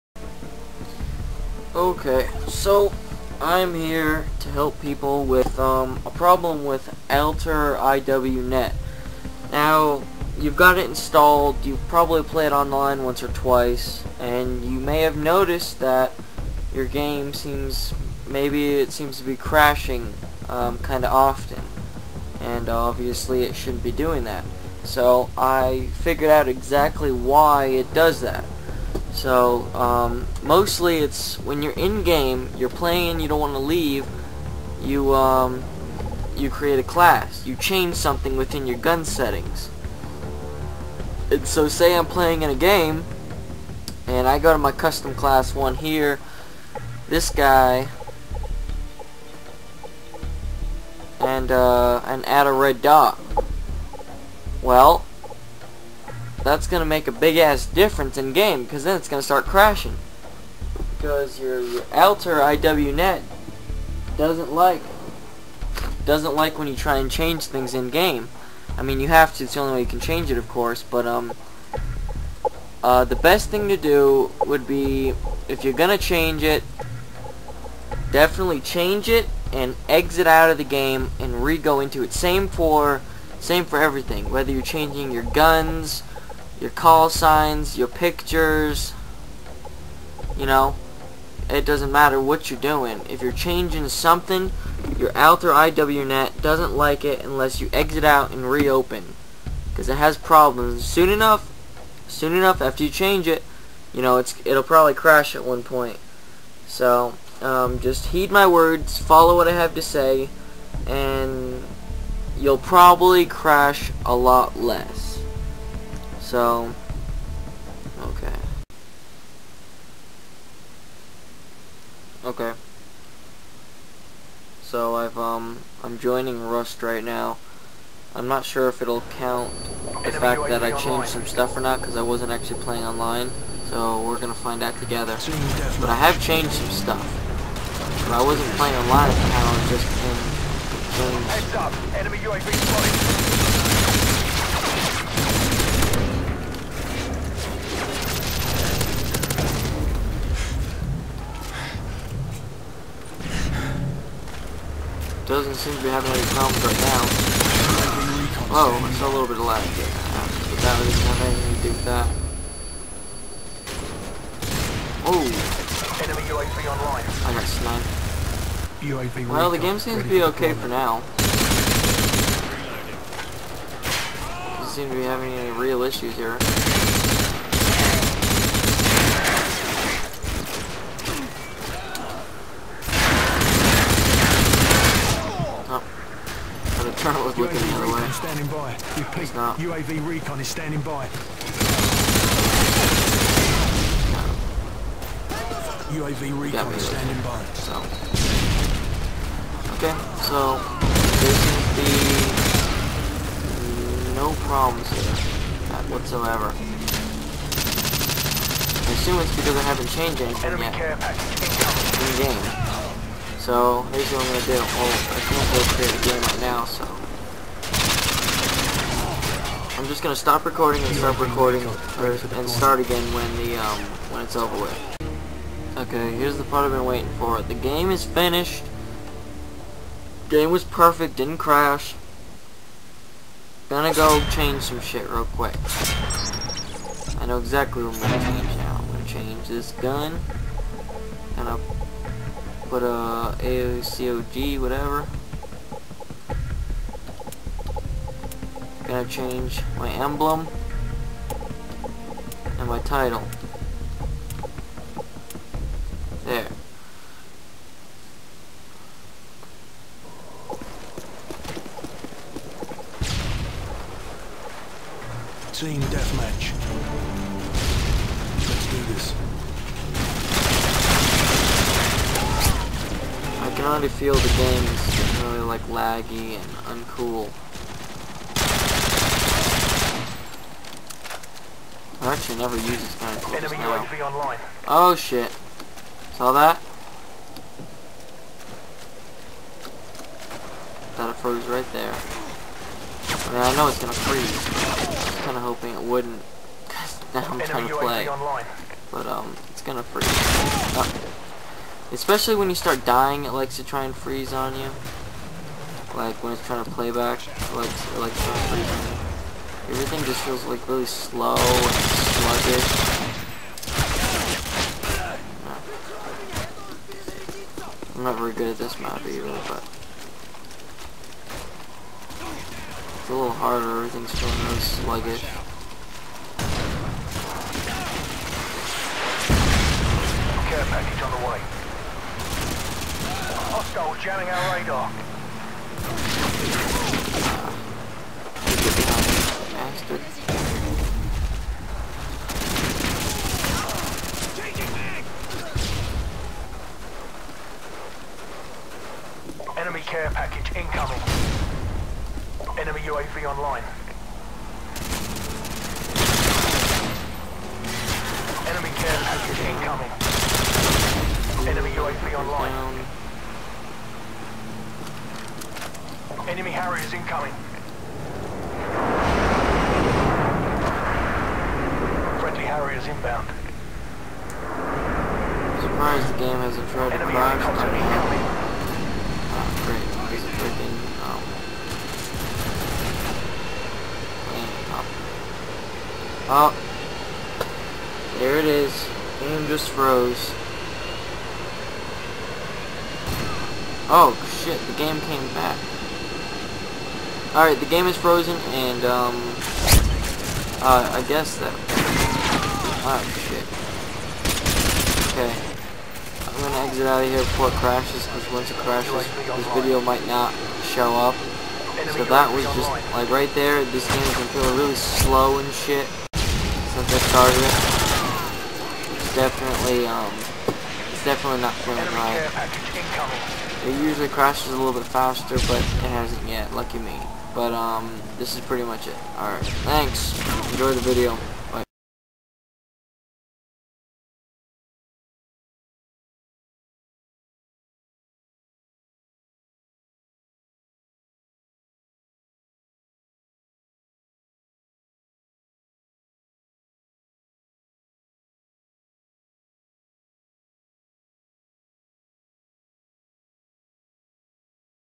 alteriwnet crashing {FIX} most errors
this is a video i made to help people understand why sometimes they can play for a while without trouble, and fix when it starts freezing and crashing.